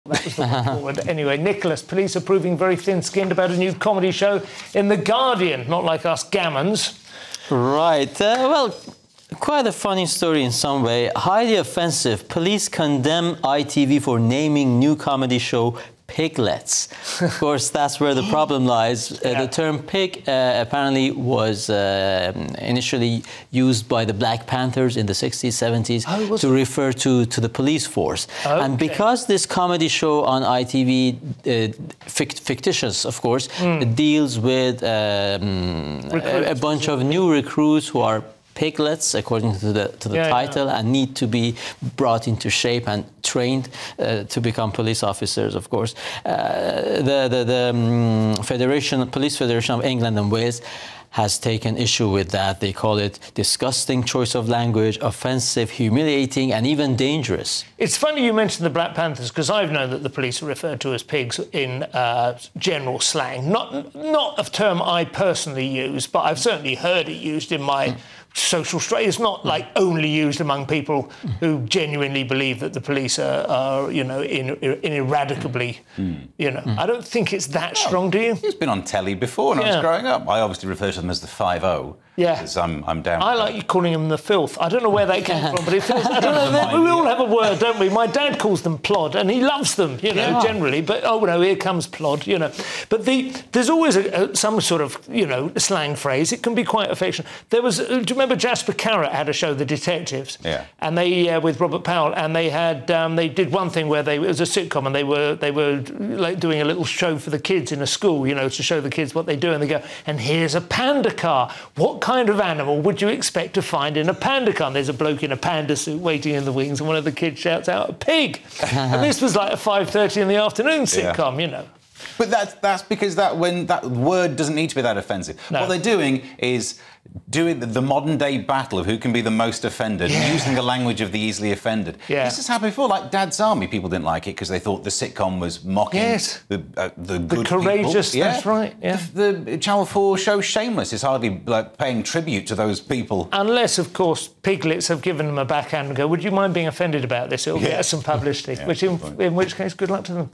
ITV BLASTED by 'thin skinned' Police for 'DISGUSTING' new show about cops
'I like calling them the filth'.

'Highly offensive': police condemn ITV for naming new comedy show Piglets | Guardian